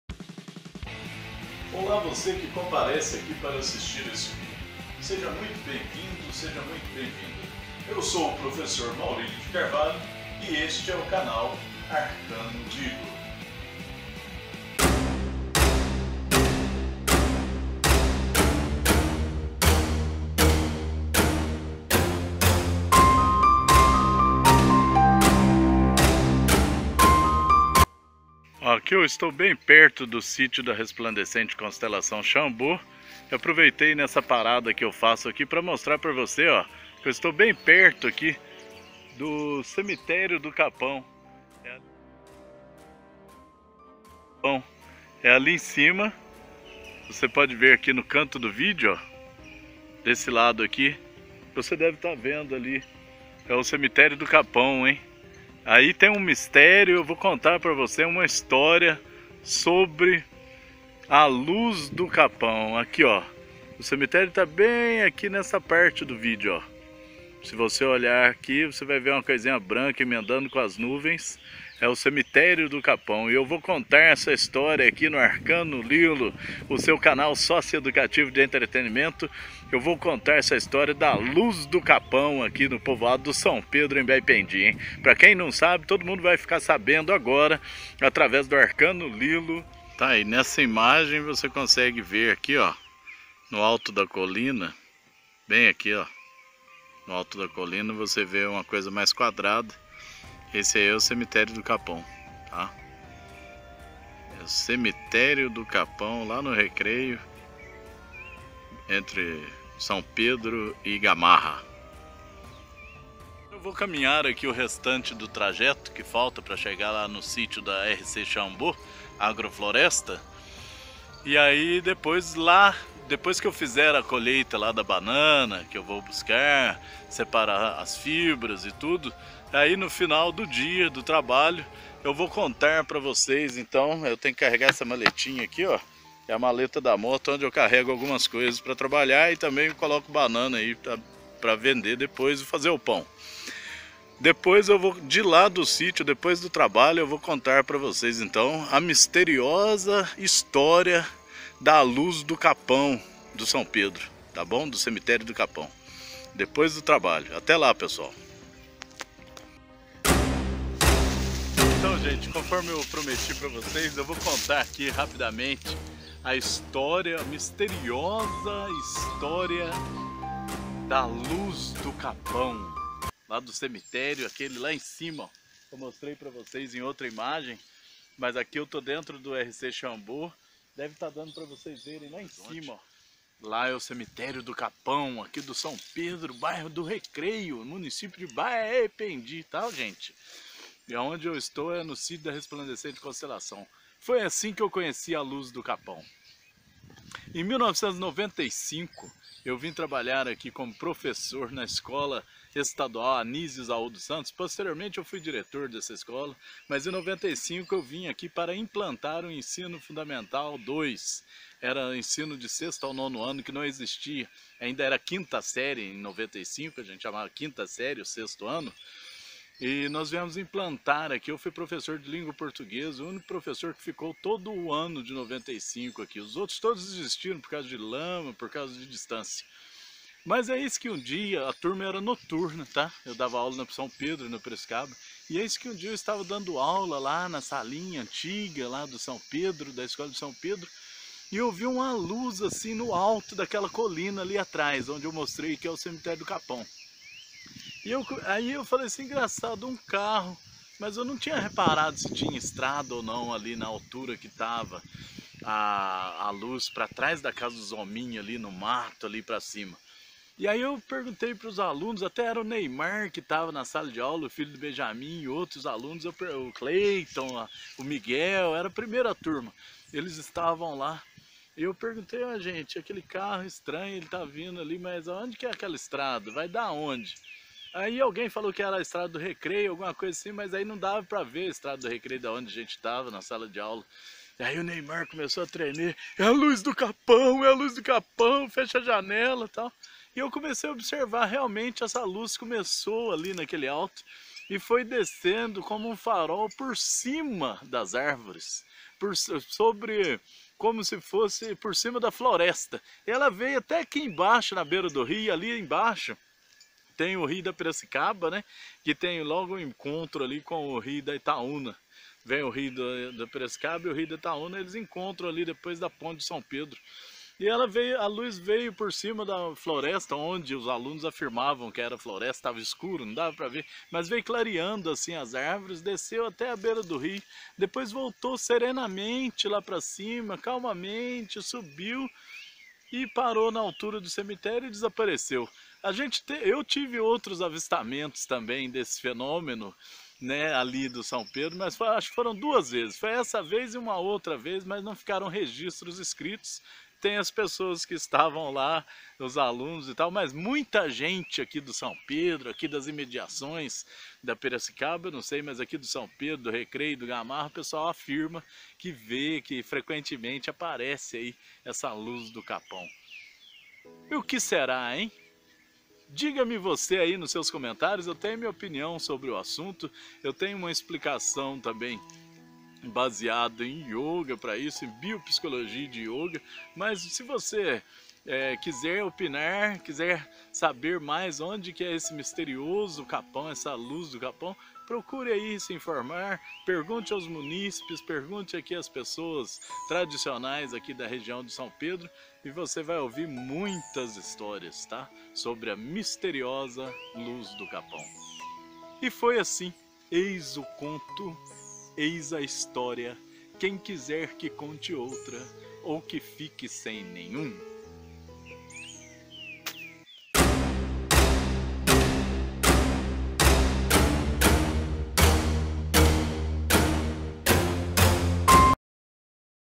A MISTERIOSA LUZ DO CAPÃO
Contação de histórias é uma das temáticaS recorrentes aqui do canal ARCANOLILO .
E neste vídeo eu apresento um "causo real" que aconteceu comigo e com alguns alunos, idos de 1995 no Povoado de São Pedro, município de Baependi/MG - "A misteriosa luz do capão" - espero que gostem se divirtam e também se perguntem: essa luz existe mesmo ou é só um causo? E se existe, o que ela é? Divirtam-se!
Curtam, compartilhem, Inscrevam-se e façam seus comentários.